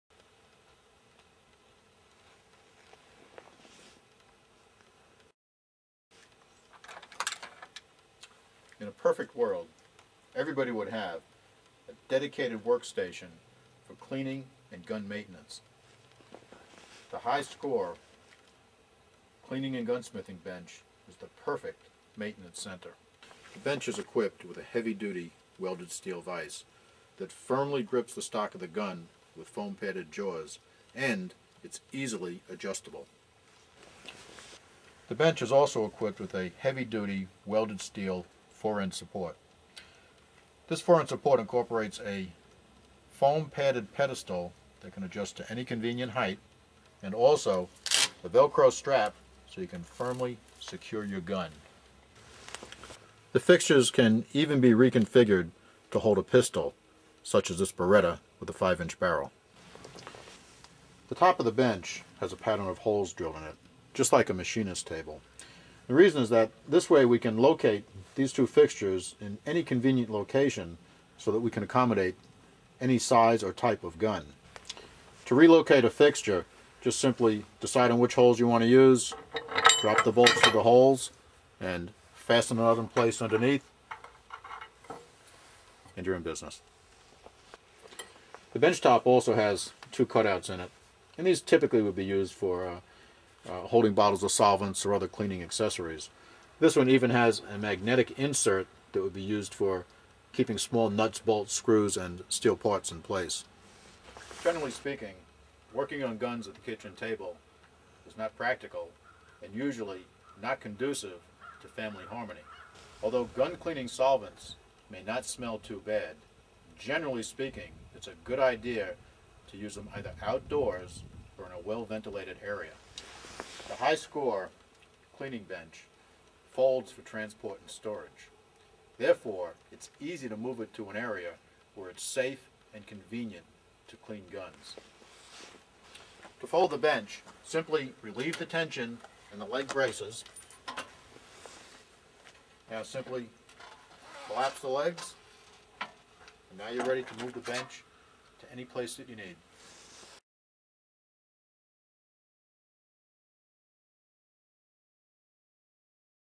Hyskore Gunsmith Bench
Folds flat 23 5/8" x 53" x 6" for storage. unsmith Bench provides a dedicated work station for gun repair and maintenance. The work surface is a comfortable 40" high and has heavy duty steel fixtures for securely clamping guns in any variety of positions. The fixtures will hold most handguns with 5" or longer barrel and all long guns, including those with pistol grips. To get more information go to our